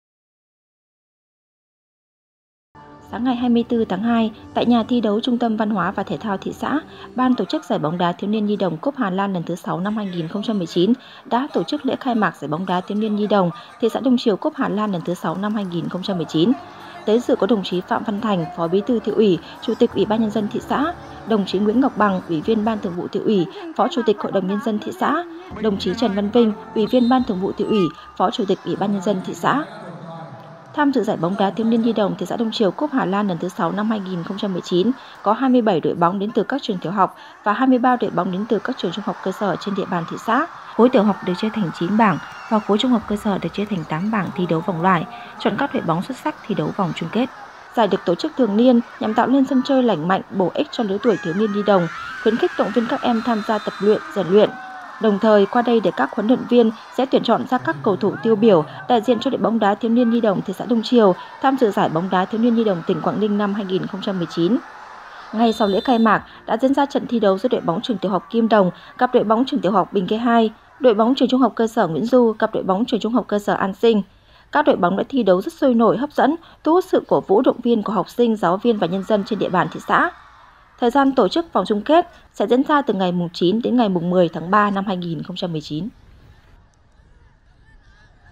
Khai mạc Giải bóng đá Thiếu niên - Nhi đồng TX Đông Triều cúp Hà Lan lần thứ VI - năm 2019
DDCI ĐÔNG TRIỀU - CẦU NỐI NGƯỜI DÂN, DOANH NGHIỆP VÀ CHÍNH QUYỀN